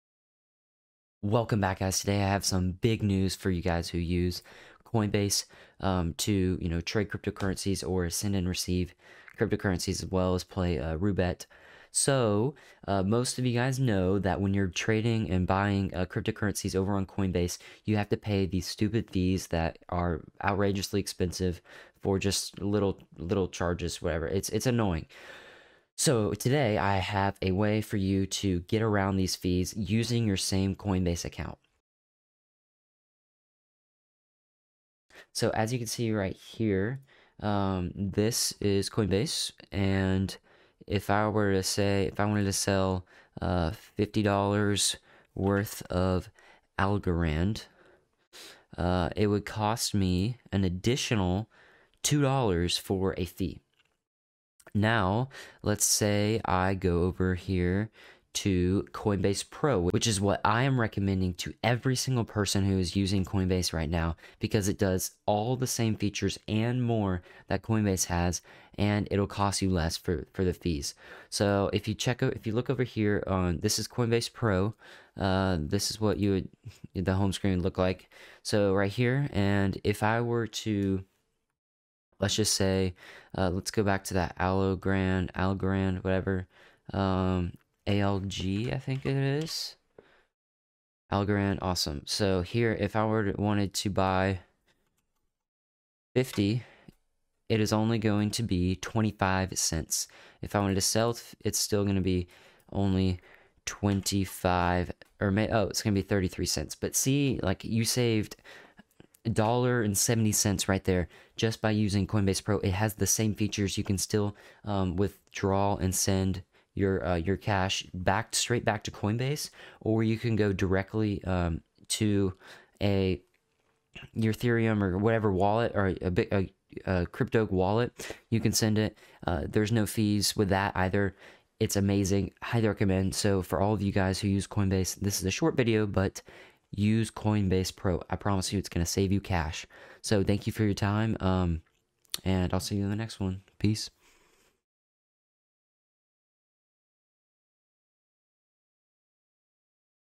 HOW TO NOT PAY COINBASE FEES
This video shows how to stop paying for the crazy Coinbase fees on every transaction. Using Coinbase pro, it will save you money and offers all the same features but more!

Need a VPN to play ROOBET? Here's 3 months FREE!